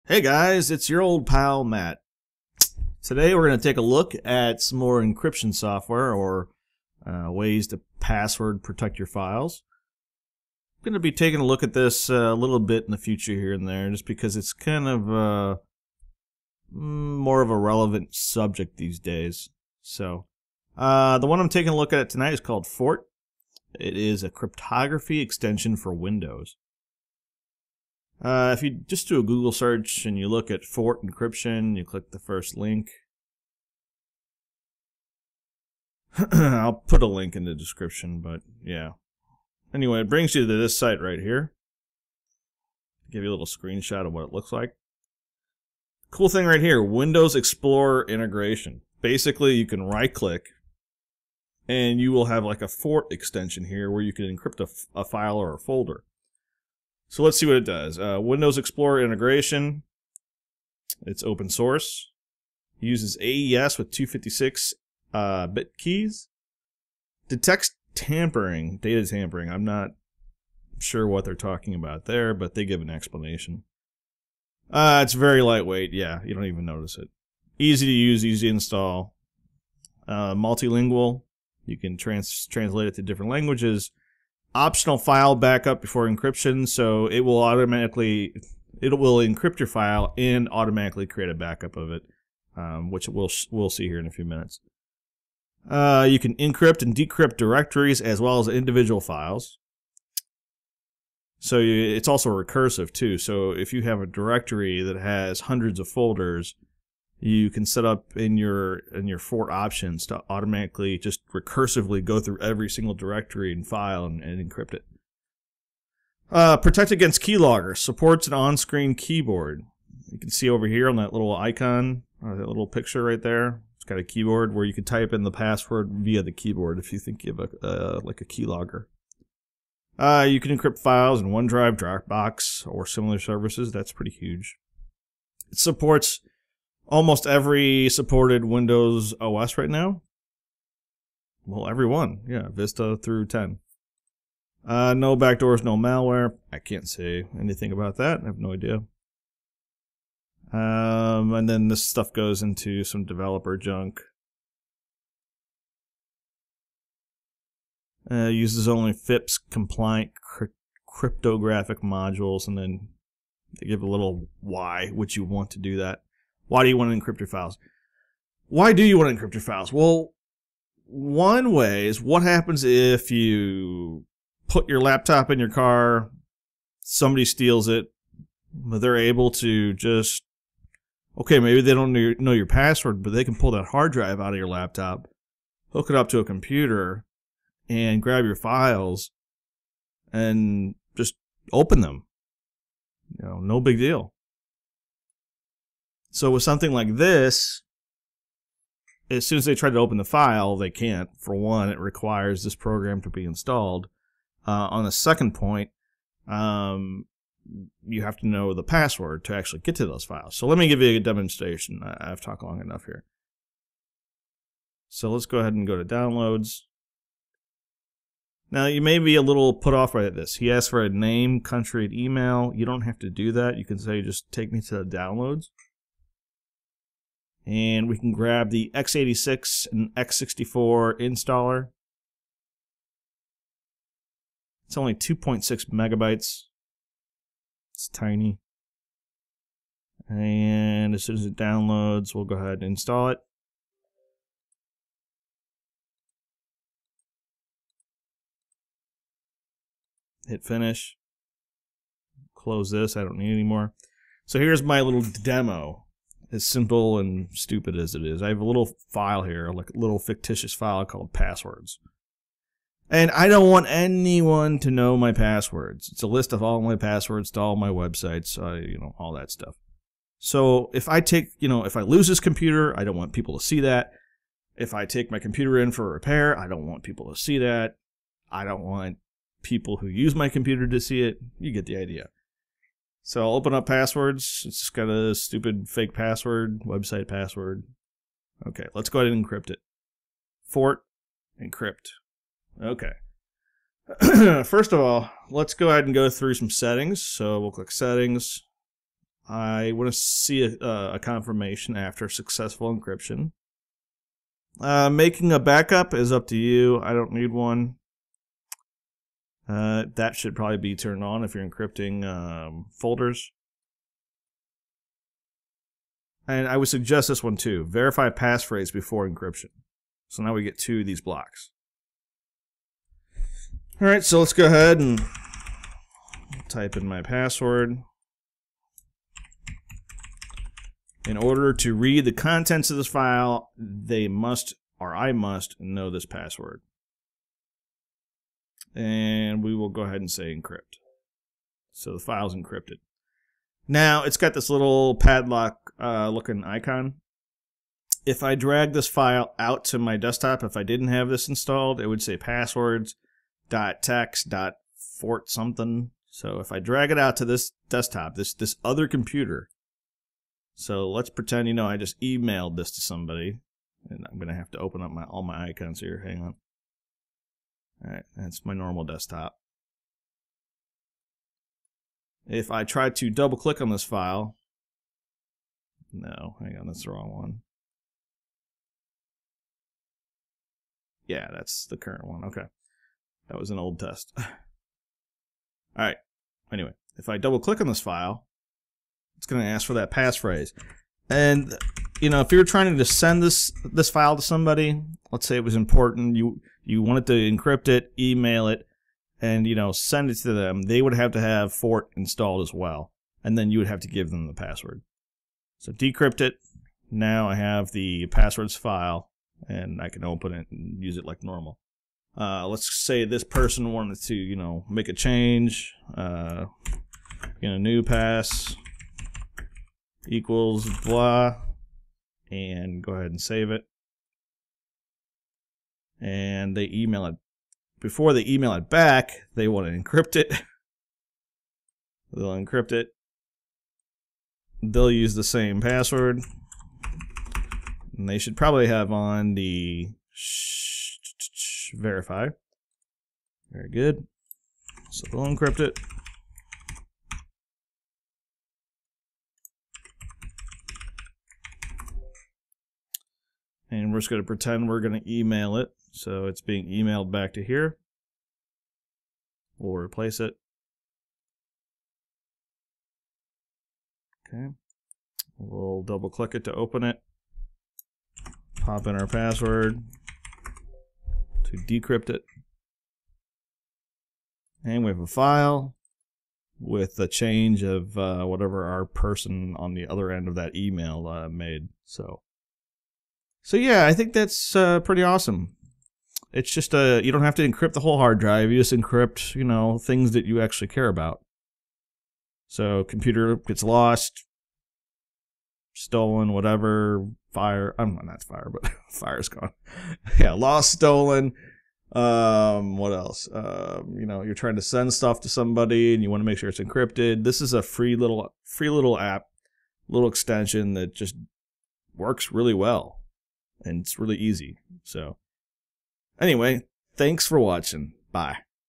Fort Encryption - Encrypt and Password Protect Folders and Files
In this video I'll show you how easy it is to encrypt and password protect your private files and folders using Fort.  Fort is a free encryption extension for Windows (vista - 10 x32 and x64).

Fort allows you to password protect folders and password protect files.